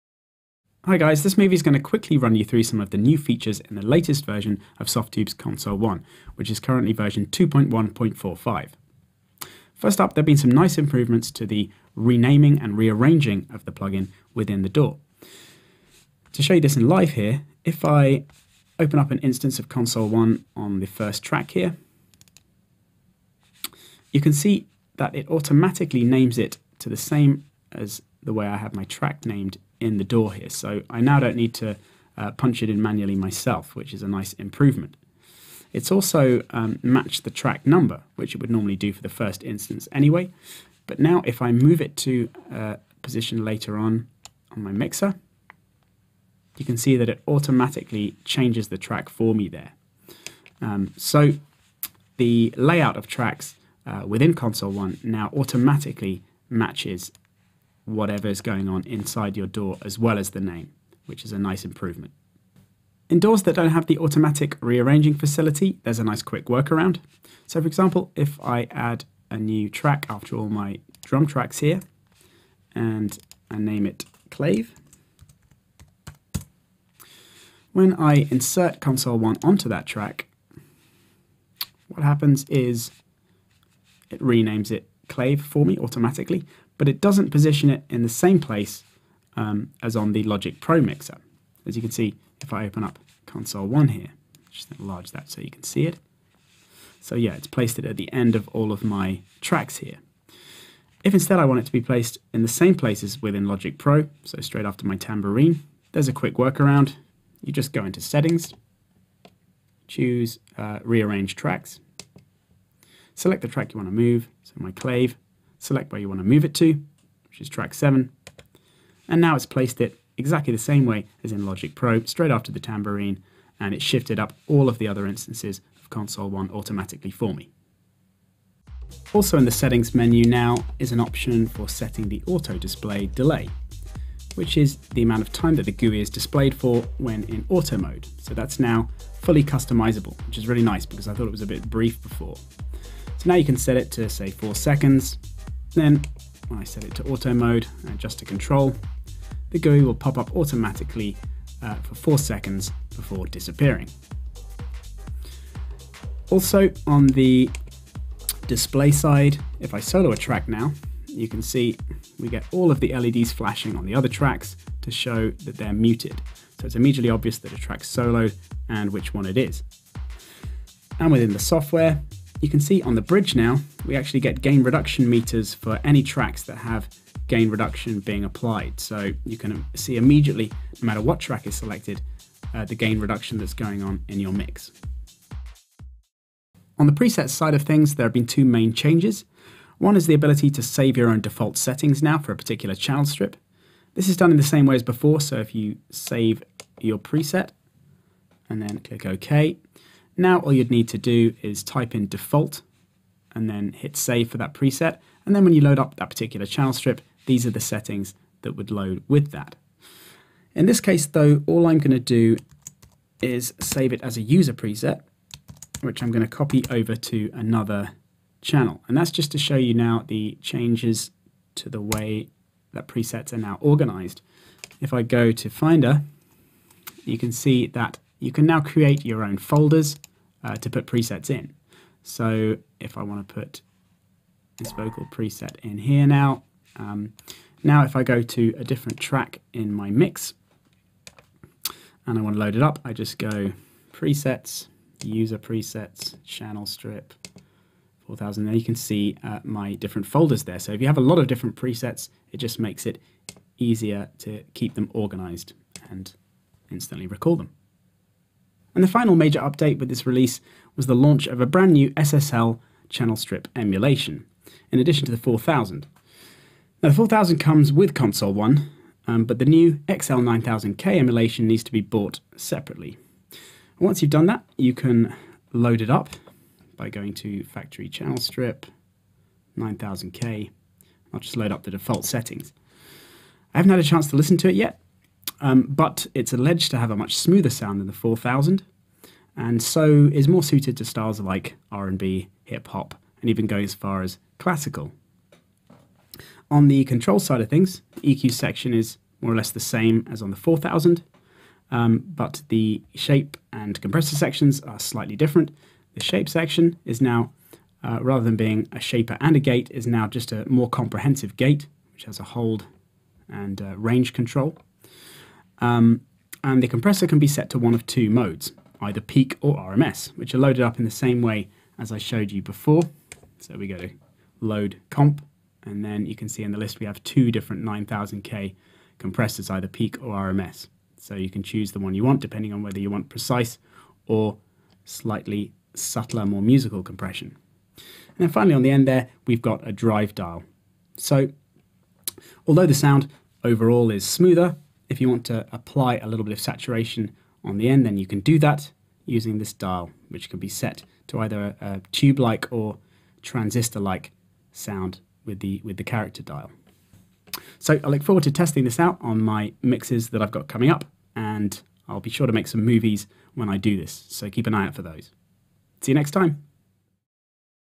New Features in Console 1 - version 2.1.45 update
A quick rundown of the new features in the latest version of Console 1.